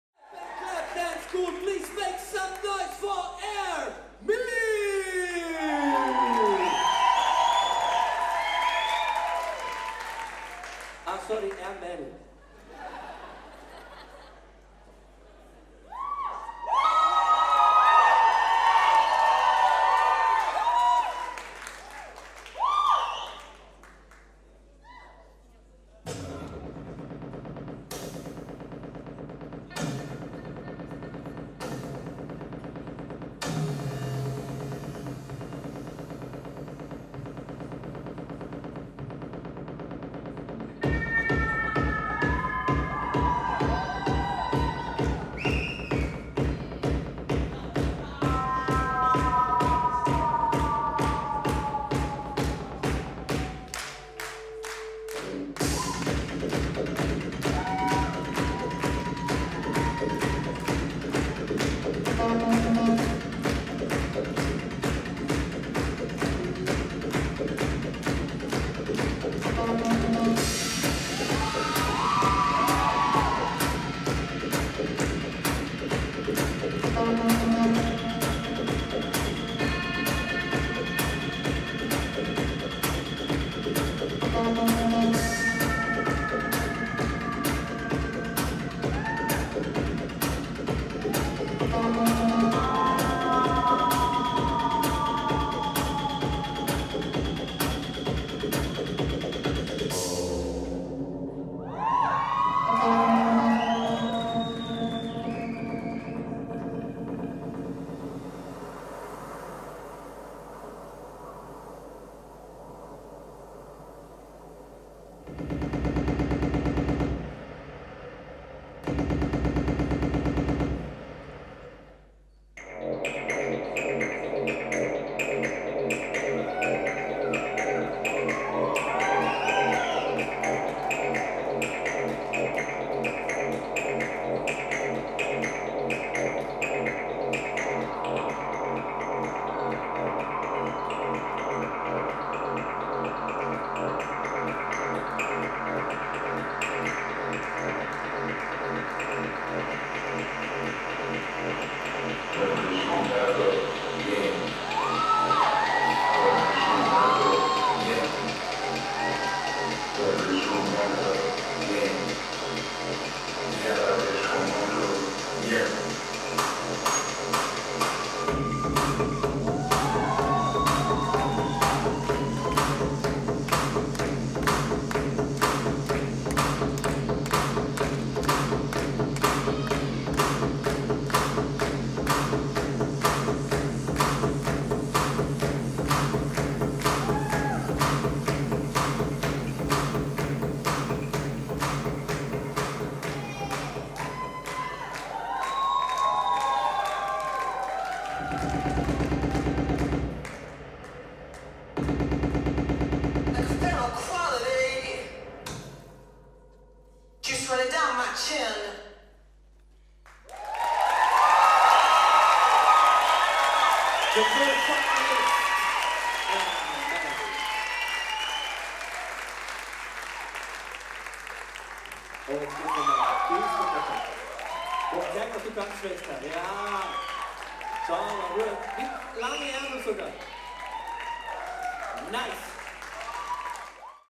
OneTwoStep DANCE Competition 2019 AIR-MEL 3. Platz
Wir machten dieses Jahr zum ersten Mal an einem Tanzwettkampf mit und gewannen den 3ten Platz. Wir sind unglaublich stolz auf unsere Tänzerinnen.

TANZ MIT UNS!

Step & Clap Tanzschule
Oberstrasse 25, 9000 St. Gallen CH
stepclap.ch